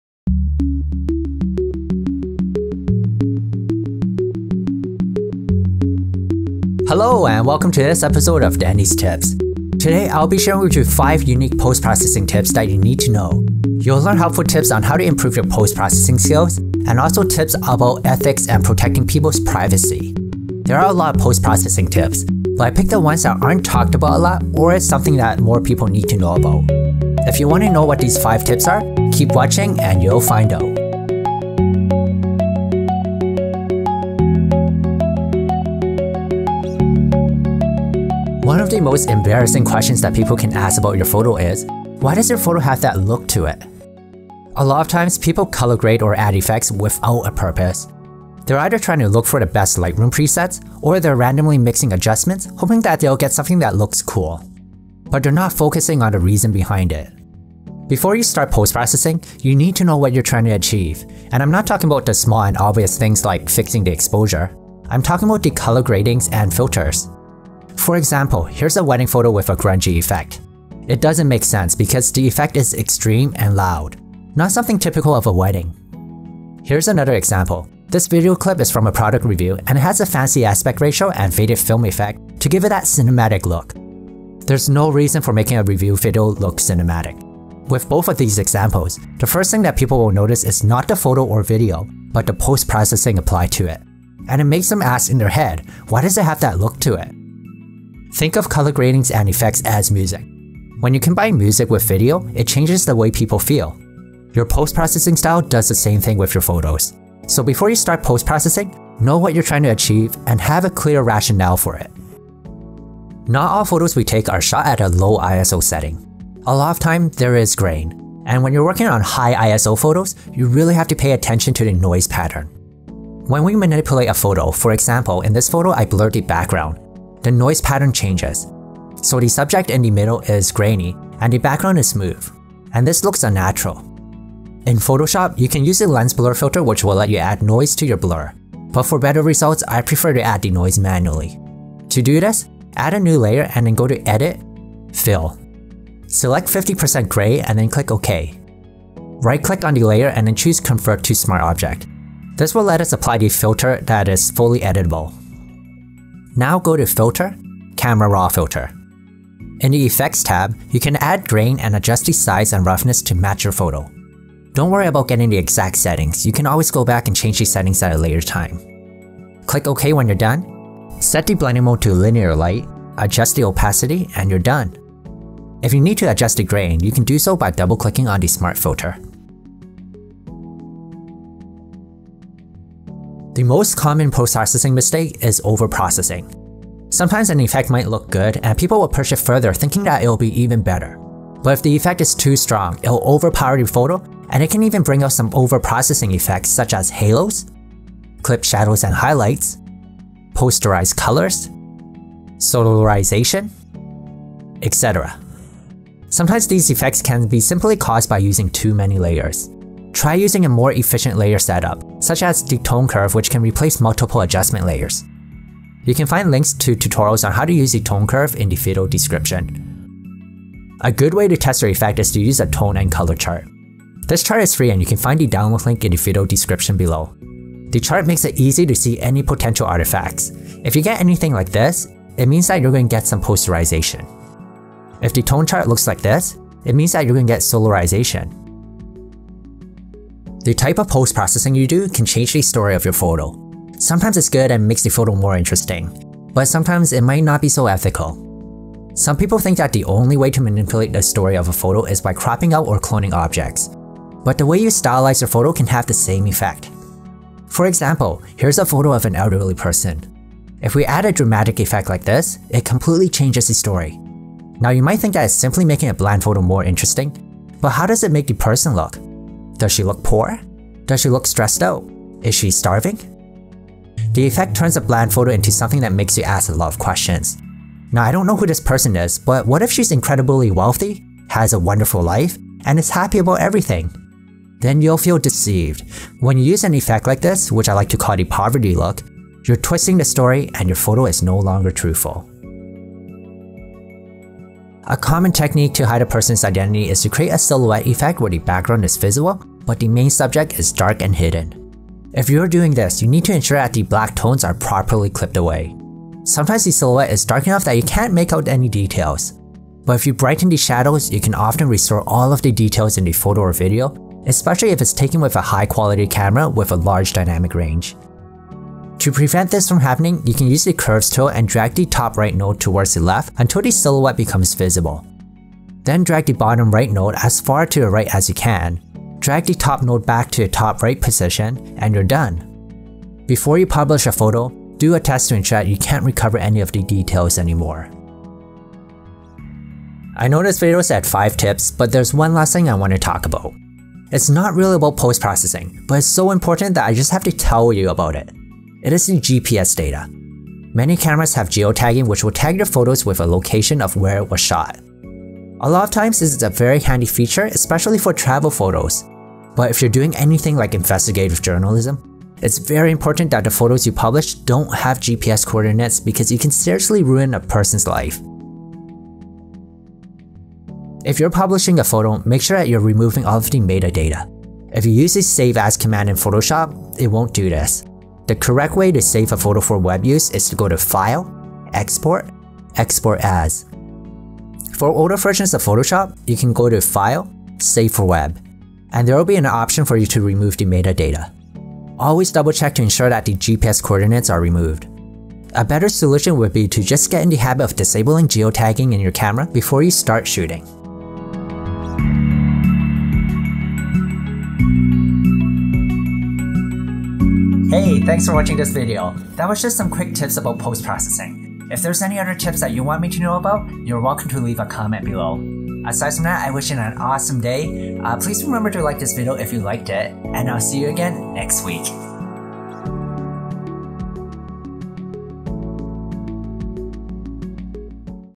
5 Important Post Processing Tips
Today, I’ll be sharing with you 5 unique post processing tips that you should know. You’ll learn helpful tips to improve your post processing skills and also tips about ethics and protecting people’s privacy. There are a lot of post processing tips but I picked the ones that aren’t talked about a lot or is something that more people need to know about.

1. Start with a Purpose
2. Maintain a Consistent Noise Pattern
3. Avoid Overprocessing
4. Is Your Photo Truthful?
5. Protect People's Privacy

📚 Photoshop Books